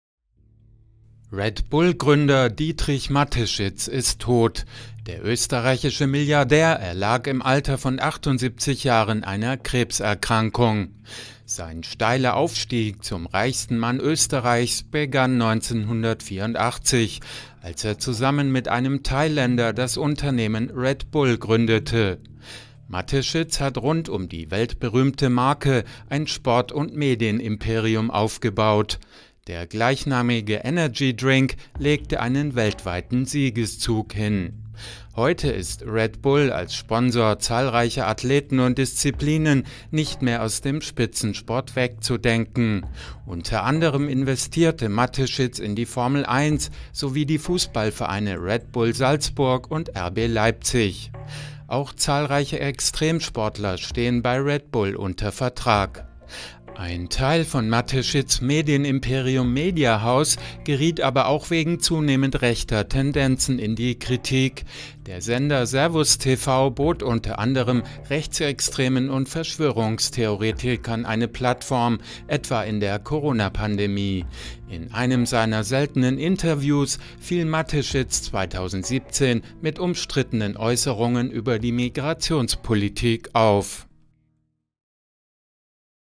Red-Bull-Gründer Dietrich Mateschitz ist tot | AFP
Red-Bull-Gründer Dietrich Mateschitz ist tot. Der österreichische Milliardär erlag im Alter von 78 Jahren einer Krebs-Erkrankung. Er hatte rund um die weltberühmte Marke Red Bull ein Sport- und Medienimperium aufgebaut.